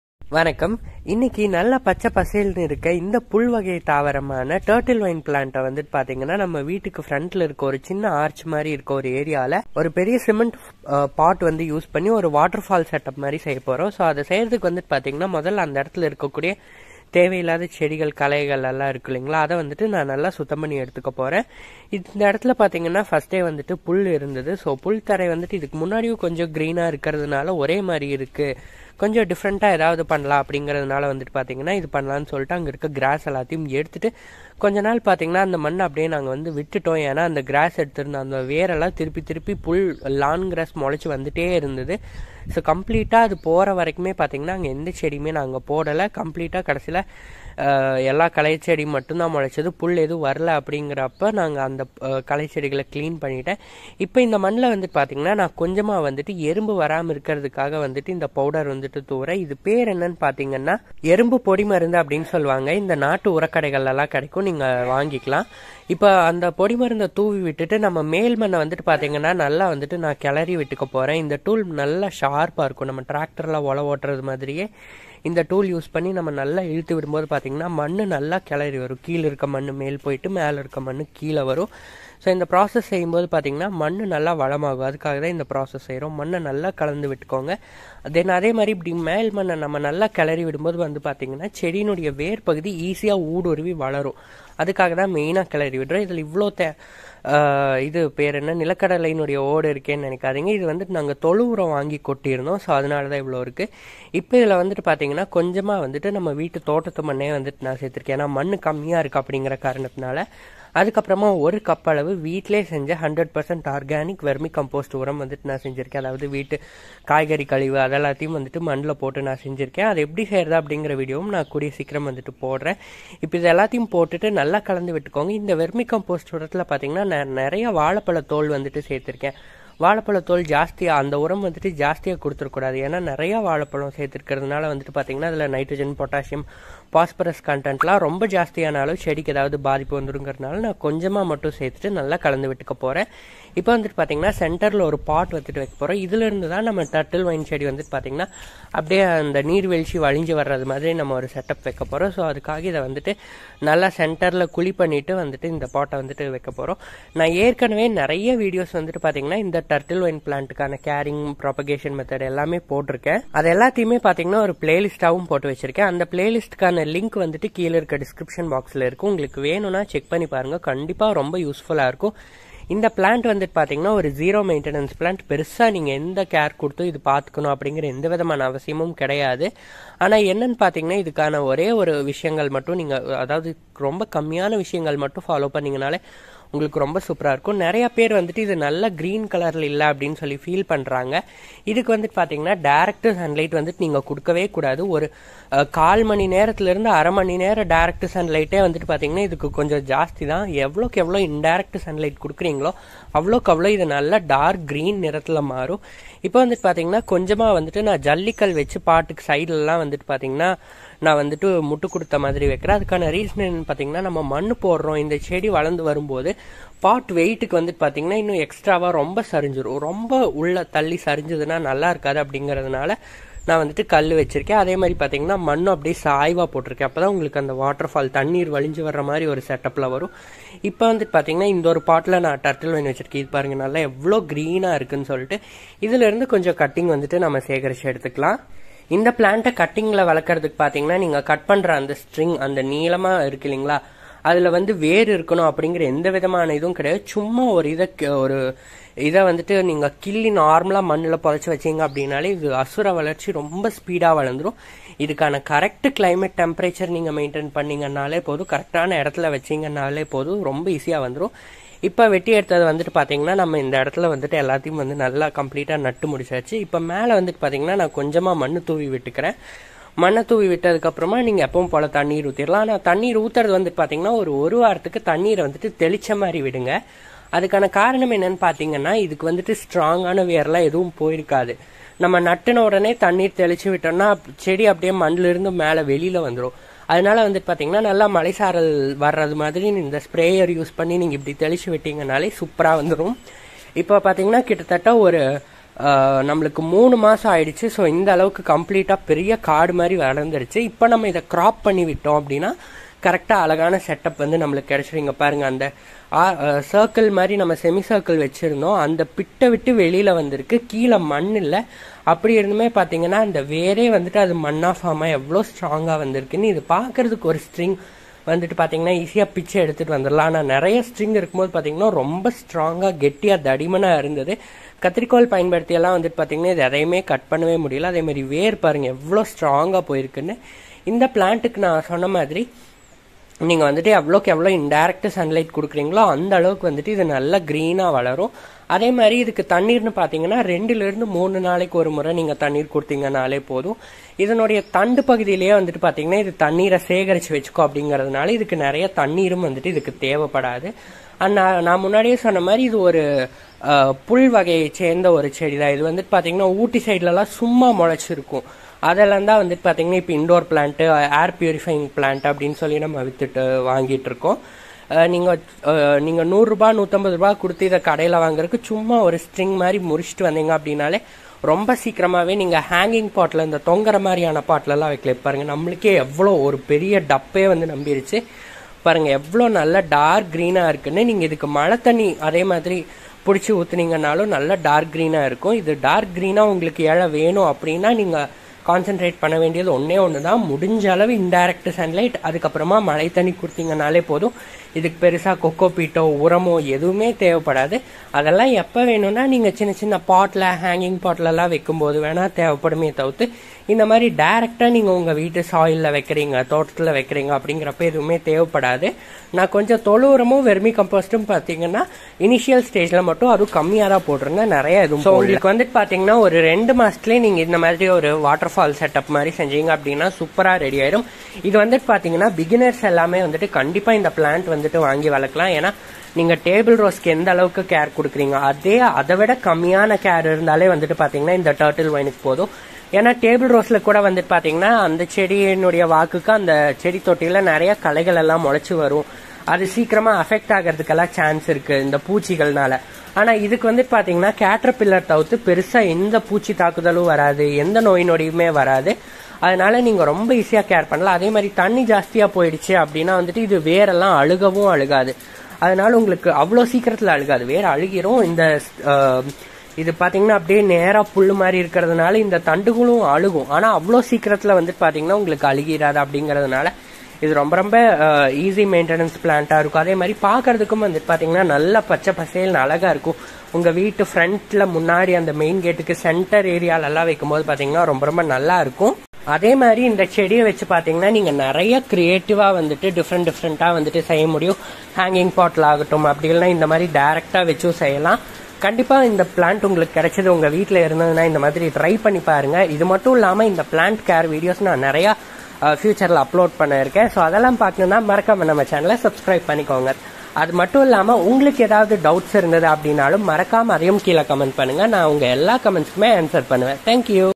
Evergreen turtle vine care | Turtle vine plant care in Tamil | Gardening ideas
Hello all,

   in this video I'm sharing how to make a waterfall turtlevine setup in your garden and some tips for evergreen turtle vine.